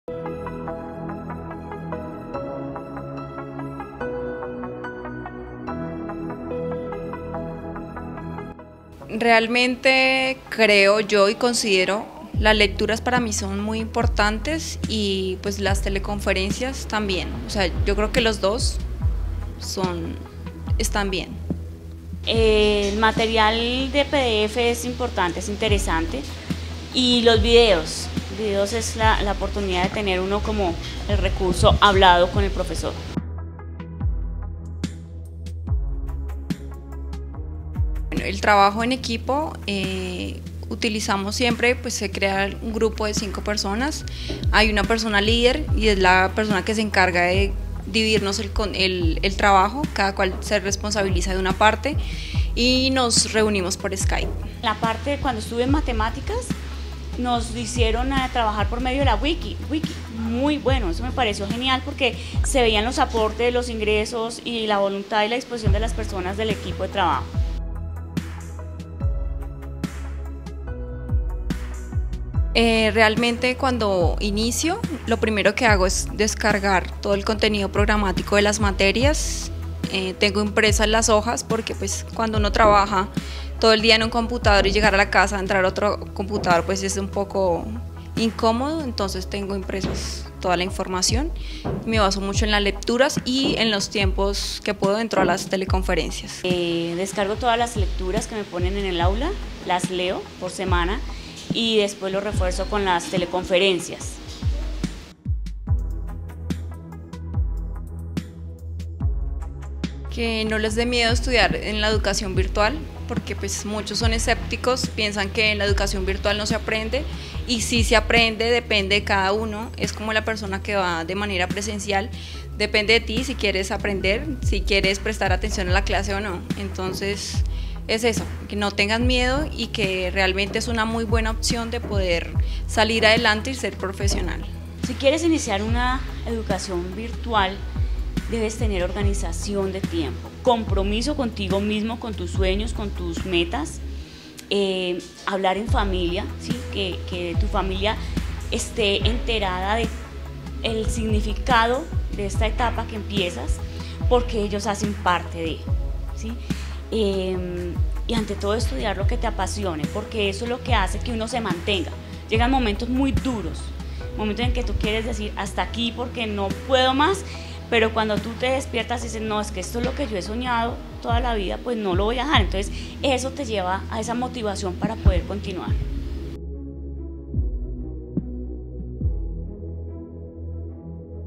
Estudiantes virtuales hablan sobre la modalidad de estudio virtual
Estudiantes virtuales del Politécnico Grancolombiano, CSU Engativá, nos cuentan su experiencia durante su preparación profesional, por medio de la educación virtual en el Poli. Entérate de más información sobre educación virtual